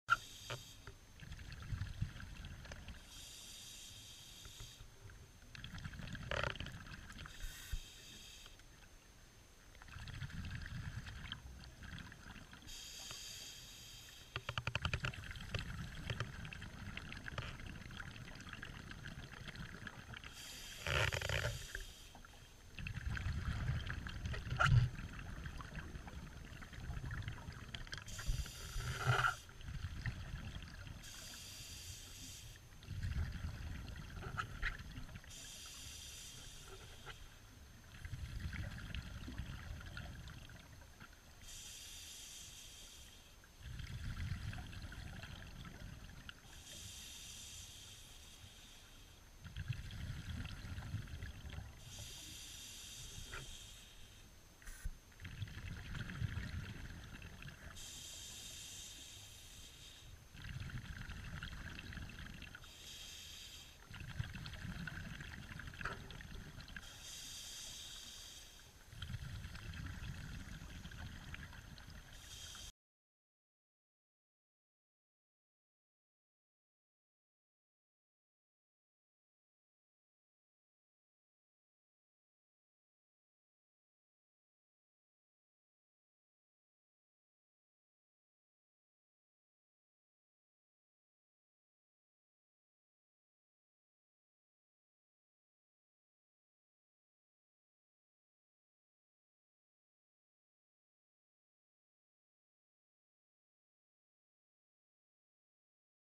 Underwater archaeology: drilling for wood sampling
A short video showing Arc-Team's divers using the Pressler drill